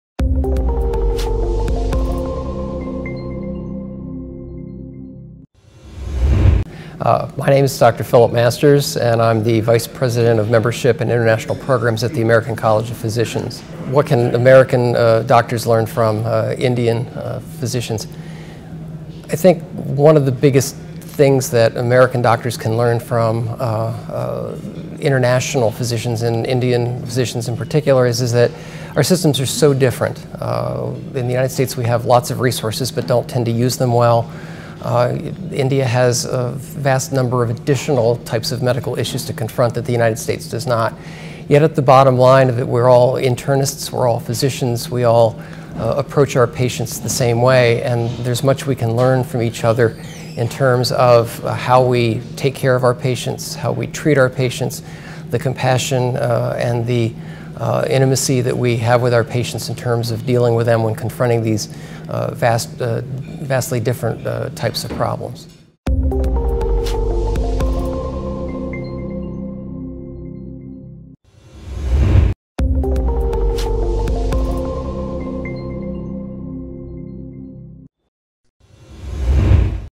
What Can American Doctors learn from Indian physicians : | Dr Philip A Masters | Medtalks
In this video, Dr. Philip A Masters, Cardiology, Vice President of Membership and International Programs at the American College of Physicians (ACP), answer "What Can American Doctors learn form Indian physicians"

Medtalks
E-219, Basement, Greater Kailash 1,
New Delhi NCR -110048 India